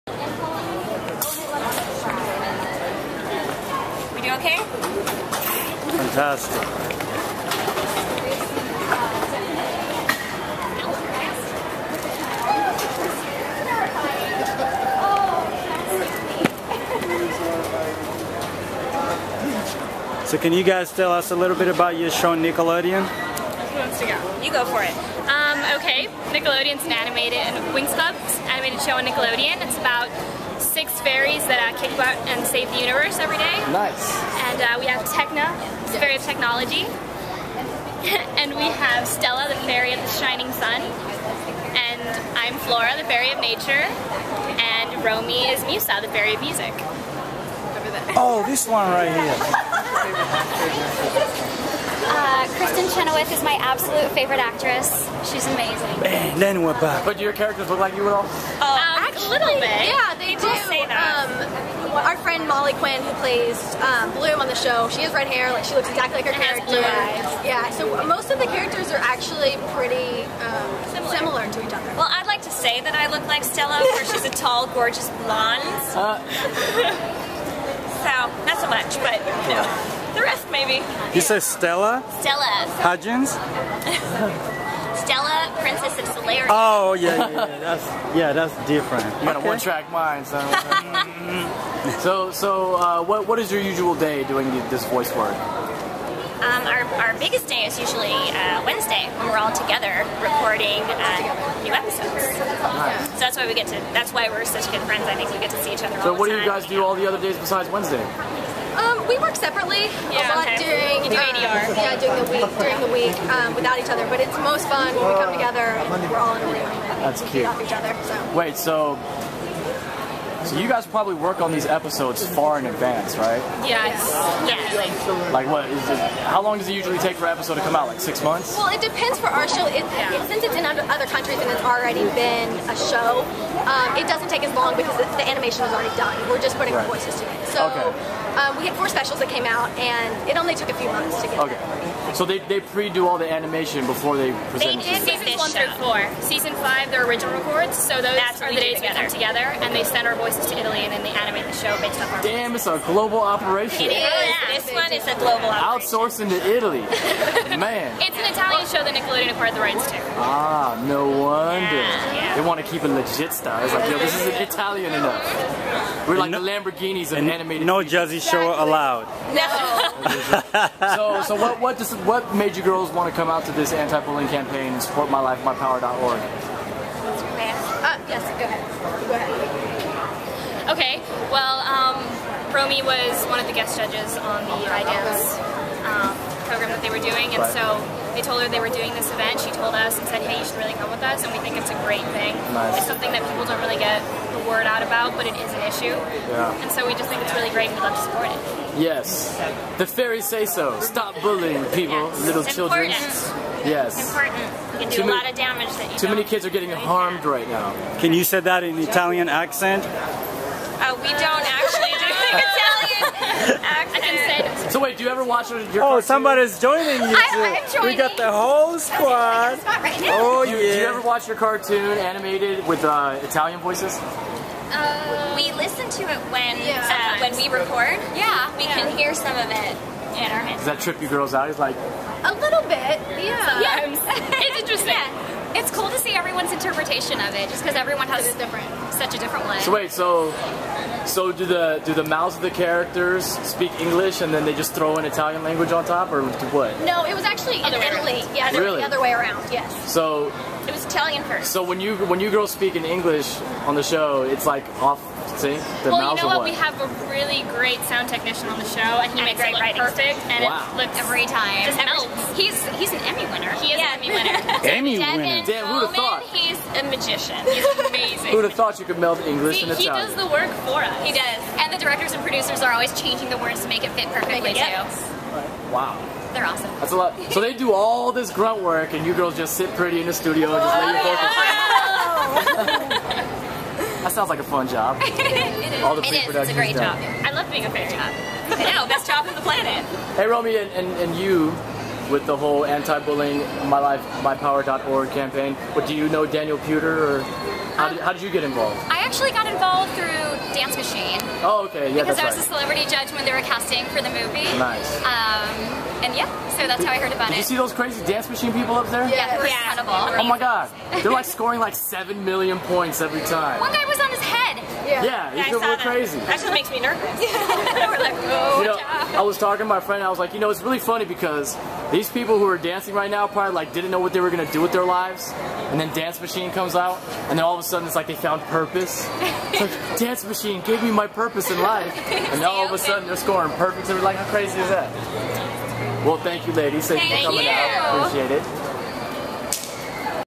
Stars of Nickelodeon Cast of Winx Club Exclusive Interview Anti Bullying Idance Machine Premiere
WTV Networks covers the stars of Nickelodeon's Wingstop. Exclusive with the stars.